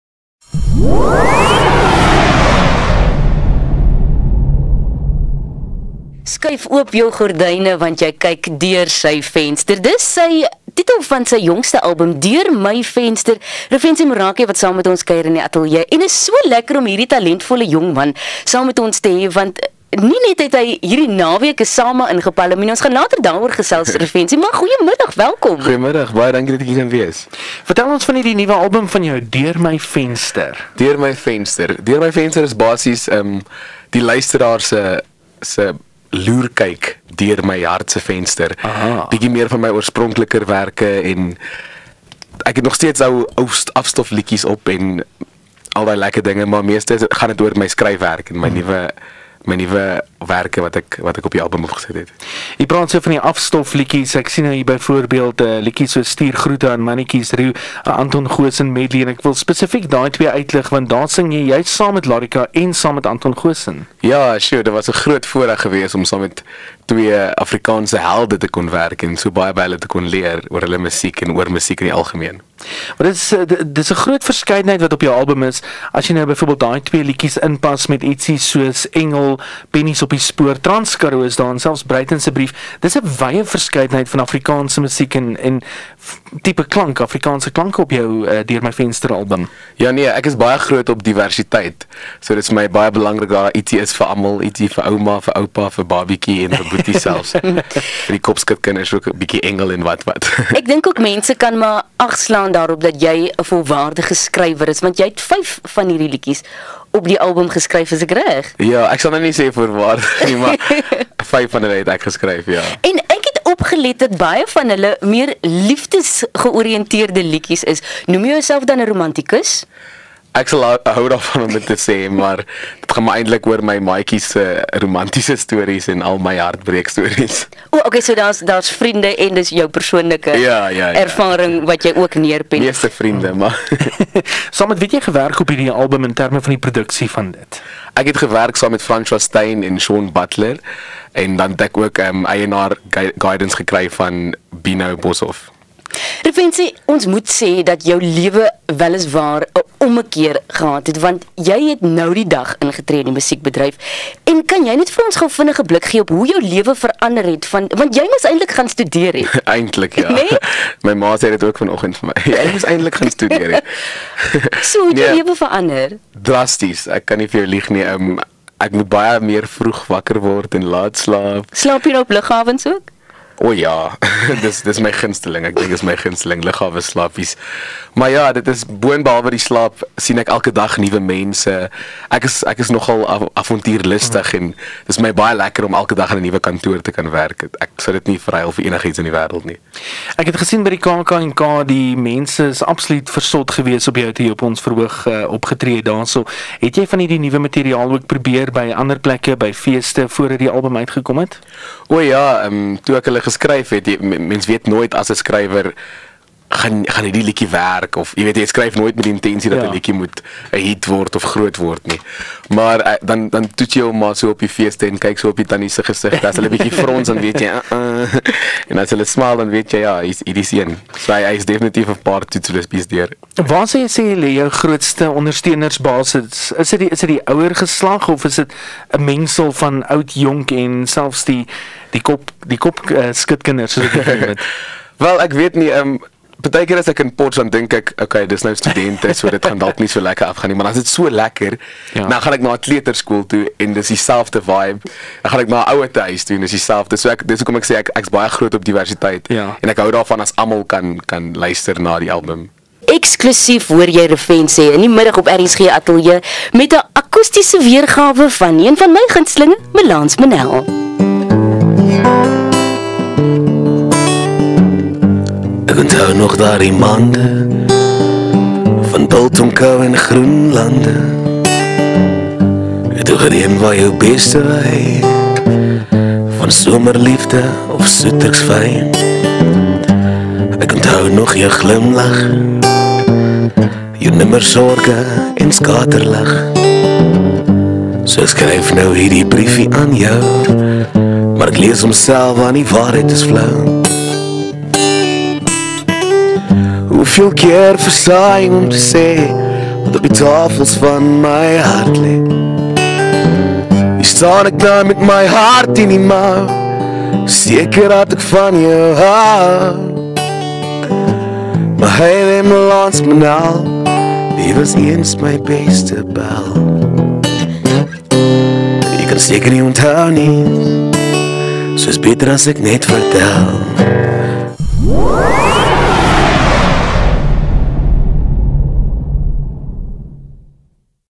Refentse
Sanger Refentse kuier by Ettienne Ludick en Sue Pyler in die Middag op RSG-ateljee.